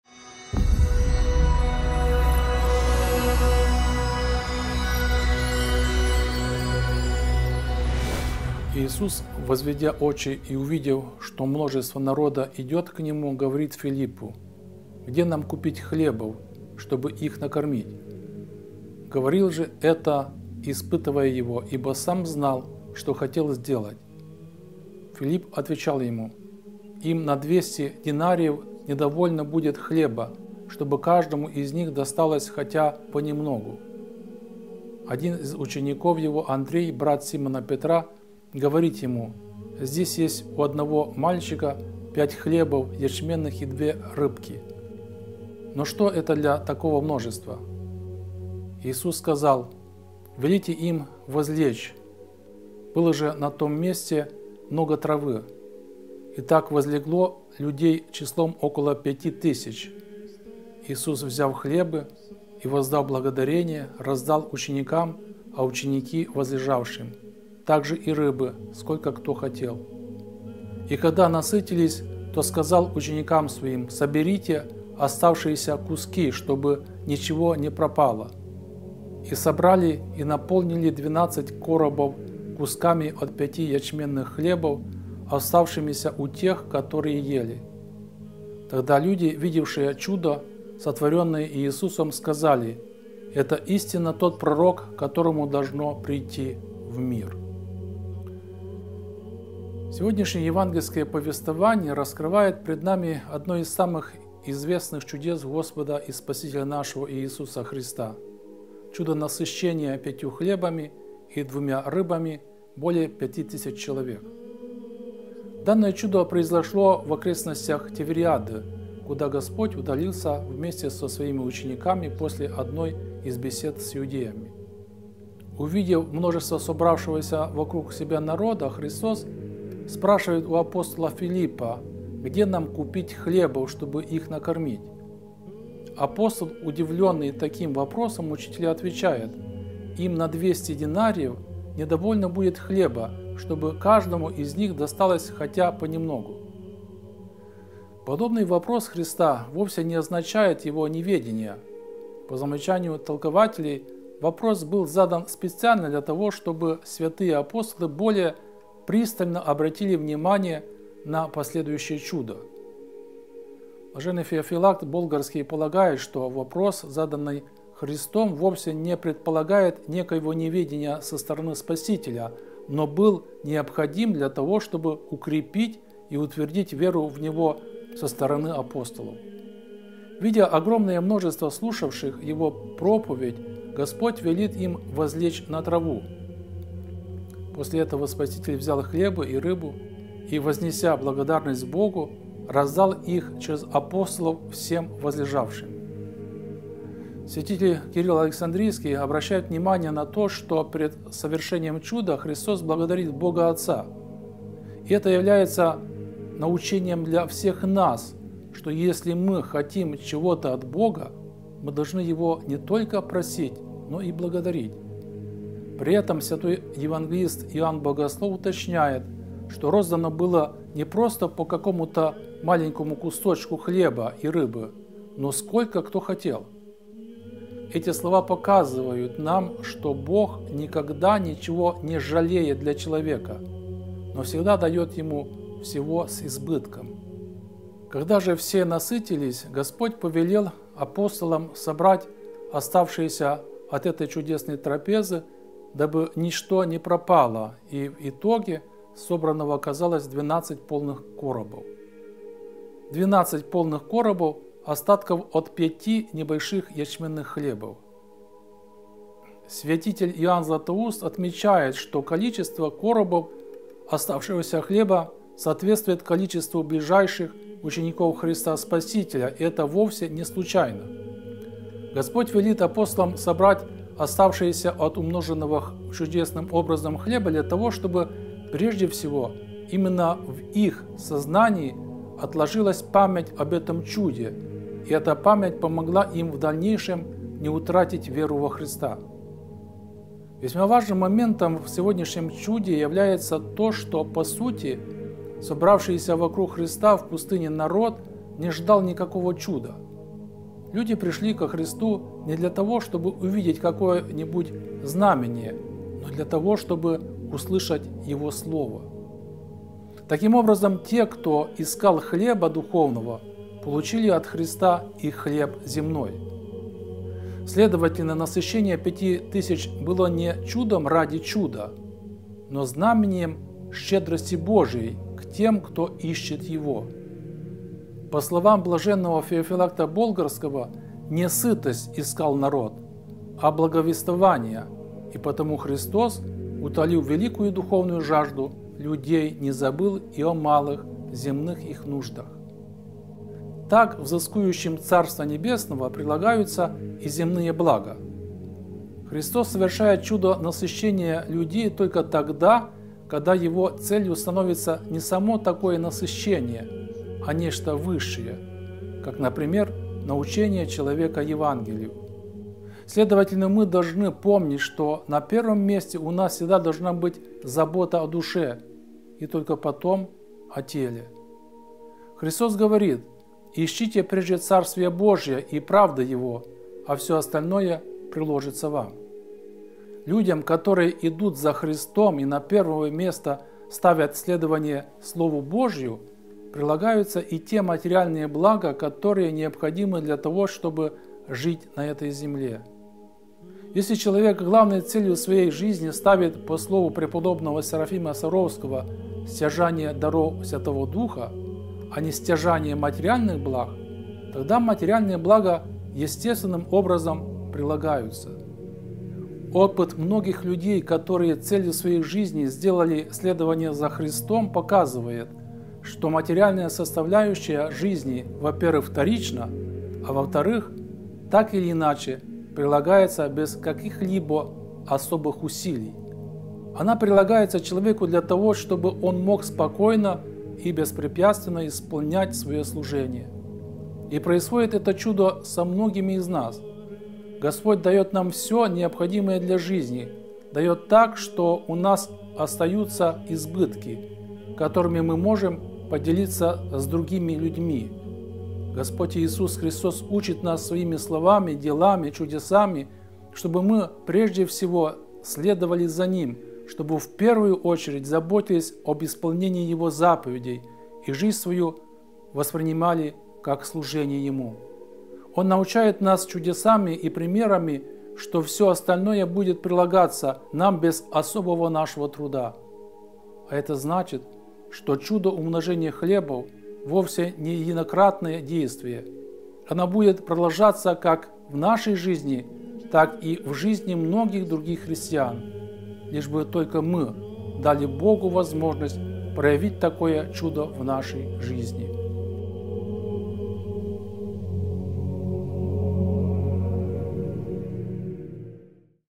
Среда, 17 мая 2023 года. Толкование Евангелия с митрополитом Антонием (Паканичем)
Сегодняшнее Евангельское повествование раскрывает перед нами одно из самых известных чудес Господа и Спасителя нашего Иисуса Христа – чудо насыщения пятью хлебами и двумя рыбами более пяти тысяч человек. Данное чудо произошло в окрестностях Тивериады, куда Господь удалился вместе со Своими учениками после одной из бесед с иудеями.

Евангелие от Иоанна 6:5-14, 18 зачало: "Иисус, возведя очи и увидев, что множество народа идет к Нему, говорит Филиппу: где нам купить хлебов, чтобы их накормить? Говорил же это, испытывая его; ибо Сам знал, что хотел сделать. Филипп отвечал Ему: им на двести динариев не довольно будет хлеба, чтобы каждому из них досталось хотя понемногу. Один из учеников Его, Андрей, брат Симона Петра, говорит Ему: здесь есть у одного мальчика пять хлебов ячменных и две рыбки; но что это для такого множества? Иисус сказал: велите им возлечь. Было же на том месте много травы. Итак возлегло людей числом около пяти тысяч. Иисус, взяв хлебы и воздав благодарение, роздал ученикам, а ученики возлежавшим, также и рыбы, сколько кто хотел. И когда насытились, то сказал ученикам Своим: соберите оставшиеся куски, чтобы ничего не пропало. И собрали, и наполнили двенадцать коробов кусками от пяти ячменных хлебов, оставшимися у тех, которые ели. Тогда люди, видевшие чудо, сотворенное Иисусом, сказали: это истинно Тот Пророк, Которому должно придти в мир".